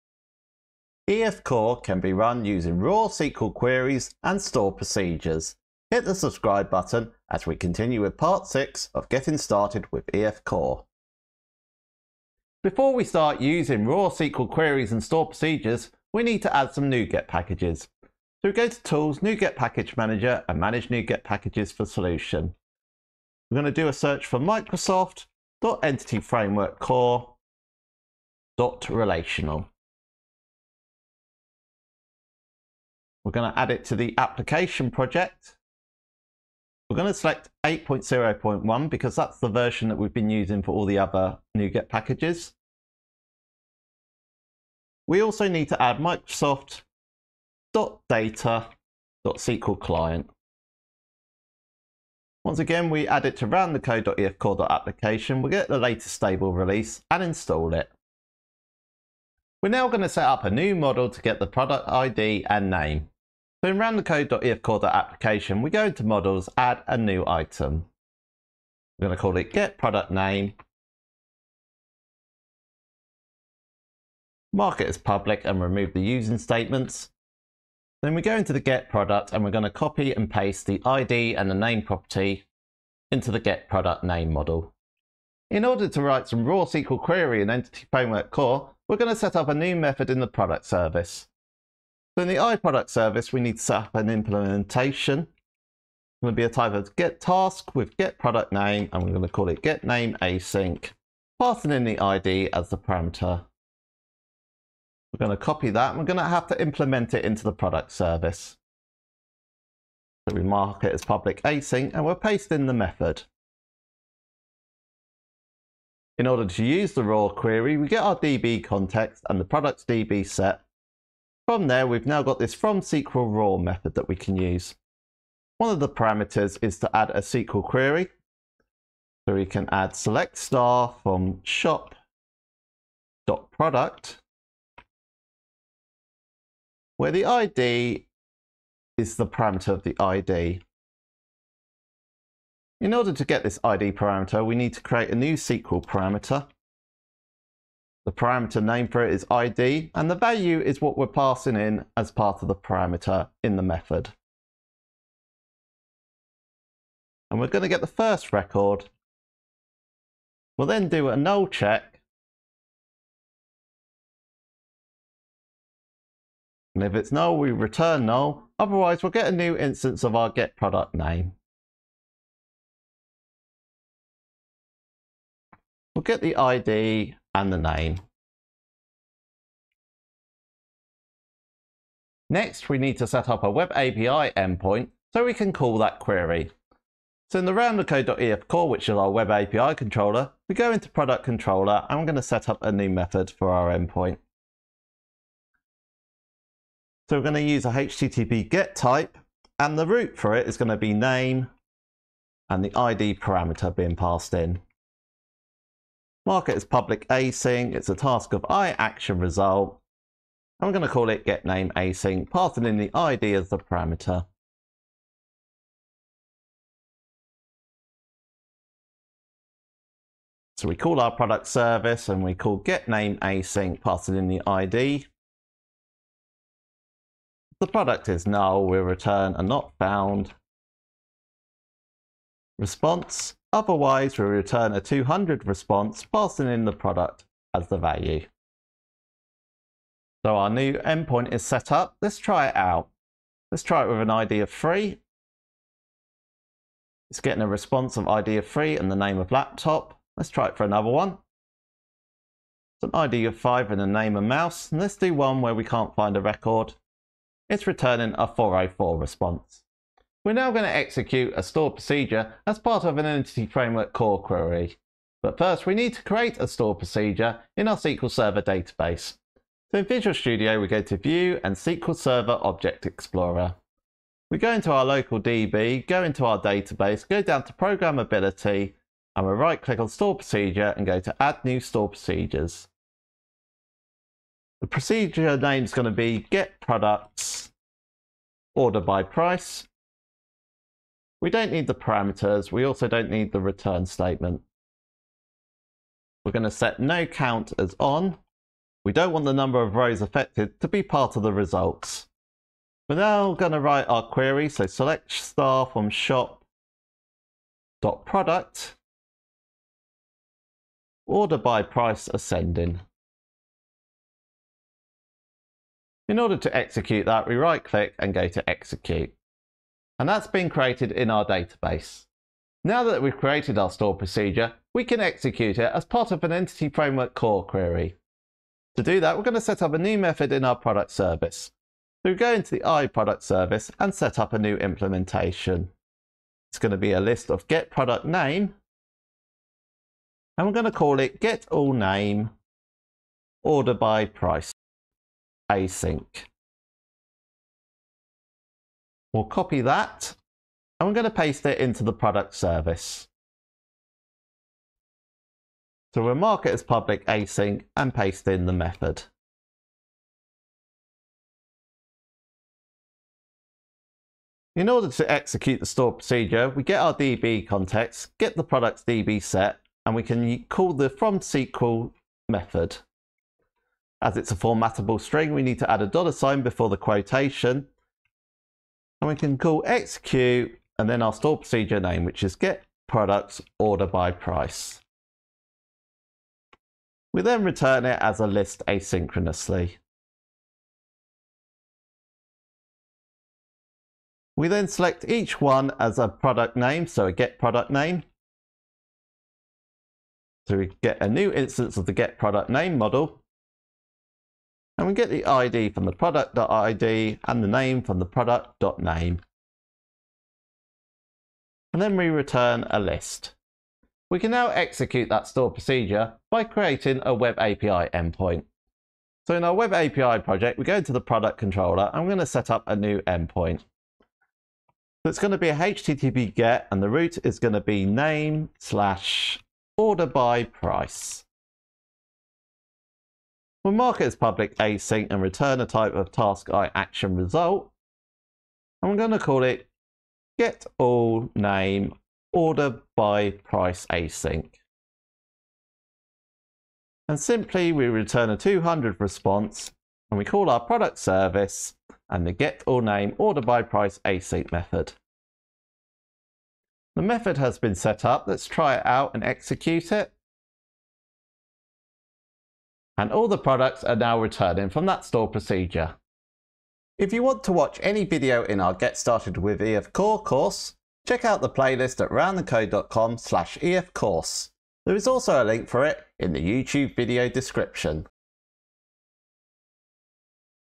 Raw SQL queries & stored procedures in Entity Framework Core
This tutorial will use an EF Core query to run a raw SQL query and execute a stored procedure from a SQL Server database.

We'll have a look at the FromSql and FromSqlRaw methods in Entity Framework Core and how they can be used to write SQL.

We'll also show you how to add a stored procedure to a context and how to add a parameter to a SQL query.

Finally, we'll execute our queries using ASP.NET Core Web API endpoints to output the results.

► This is part 6 of our Get started with EF Core course.

► Chapters
0:00 What we'll cover
0:11 Nuget packages
0:59 SQL queries
4:19 Stored procedures
7:46 Learn more about EF Core